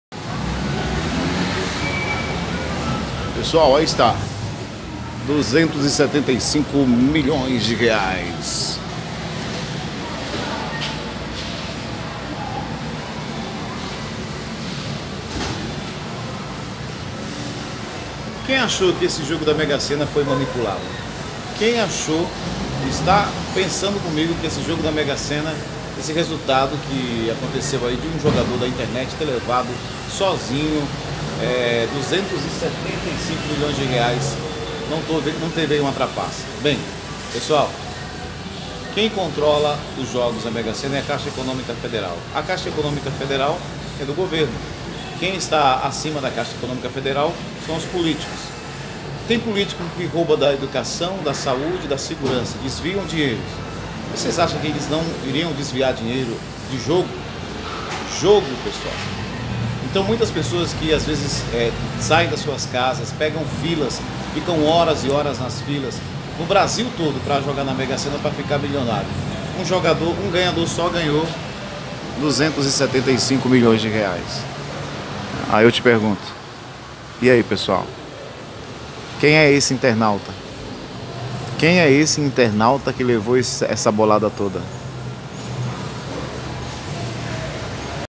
Ganhador da mesa Sena -Internet Pernambuco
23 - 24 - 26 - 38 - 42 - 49
Saiu para uma pessoa que fez o jogo online. acham que o povo é idiota. Impossível alguém acertar 6 números entre 20 e 40. Segundo Osvaldo de Souza é impossível. ainda mais online ?. Devia ser proibido. Algum político faturou.Esse jogo foi feito depois do sorteio. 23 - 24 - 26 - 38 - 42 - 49